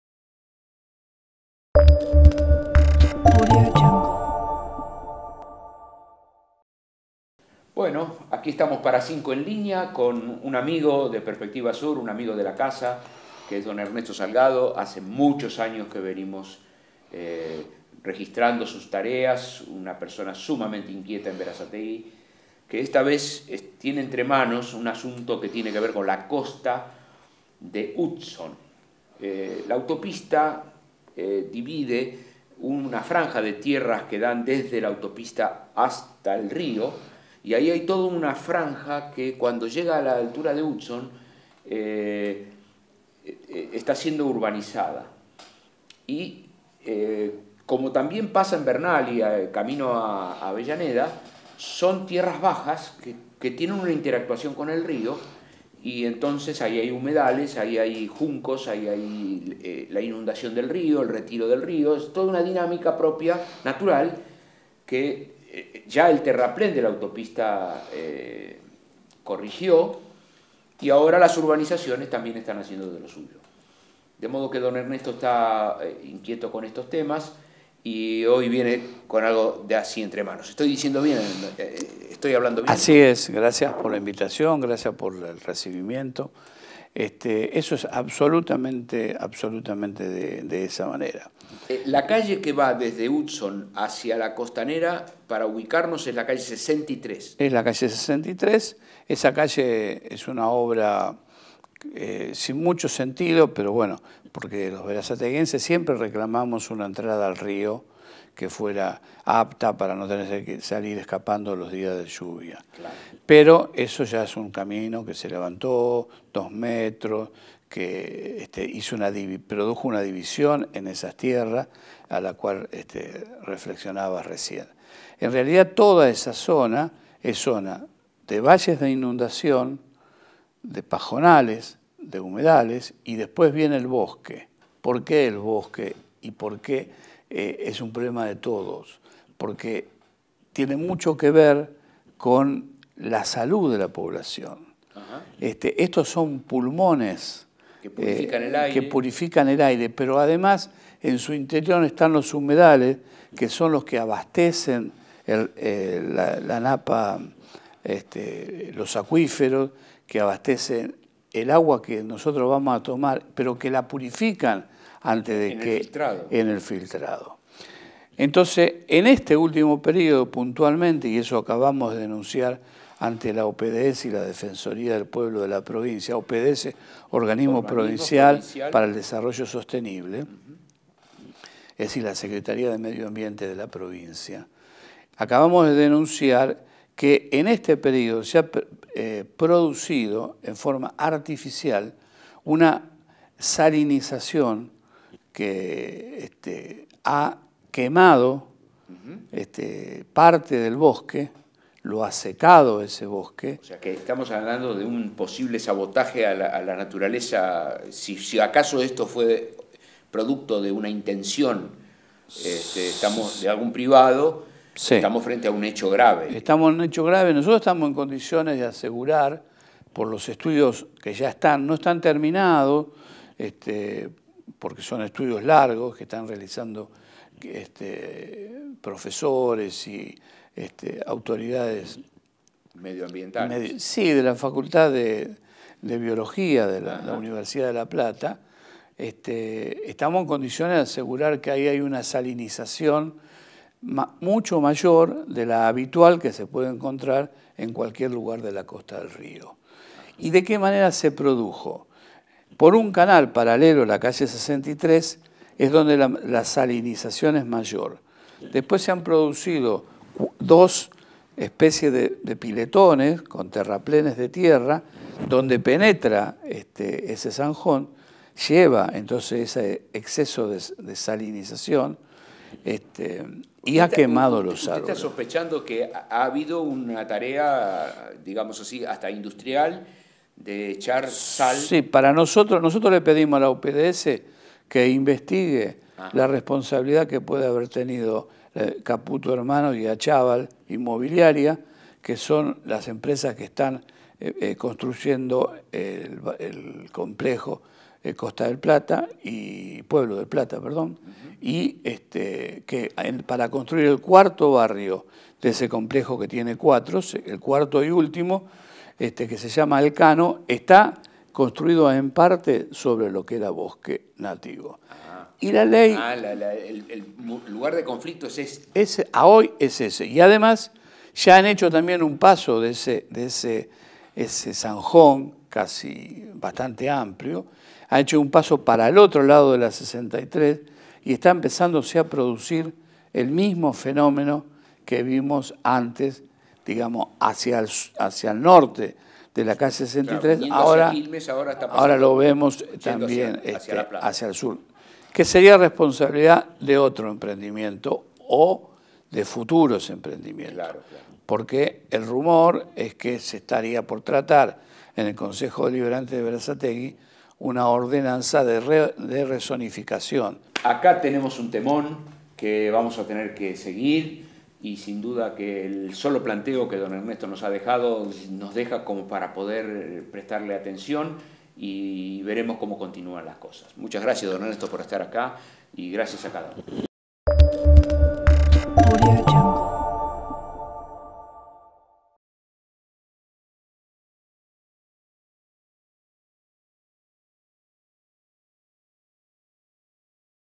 Denuncian a dos empresas por la salinización de los bosques de Hudson
El Foro en Defensa del Río de la Plata y el Medio Ambiente denunció en los últimos días ante el Organismo Provincial para el Desarrollo Sostenible (OPDS) la posible responsabilidad de dos empresas por la salinización de los humedales de la costa de Hudson, que quemó una gran cantidad de árboles de su bosque nativo.